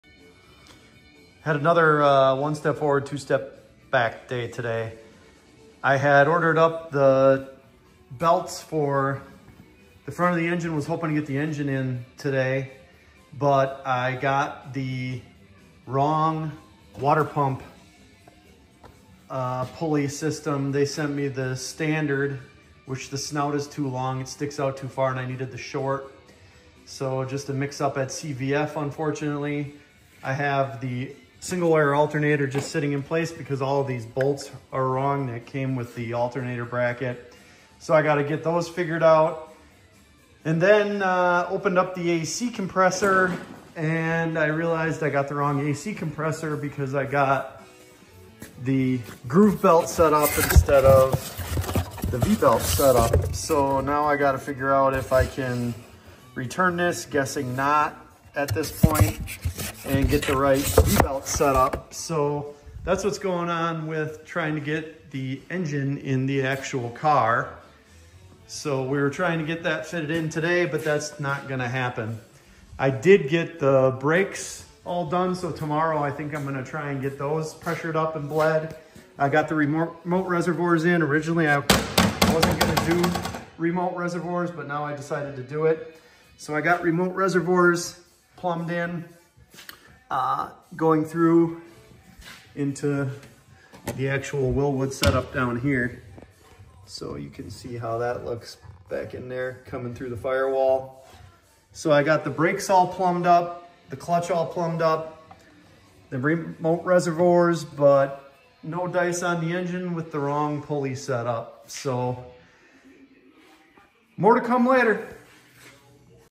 Ford GT40 (RCR40) Project: Remote brake reservoirs and front SBF pulley assembly.
Installing remote reservoirs for the brake and clutch. Attempting to finish up the front pulley assembly from CVF on the small block Ford 347 stroker.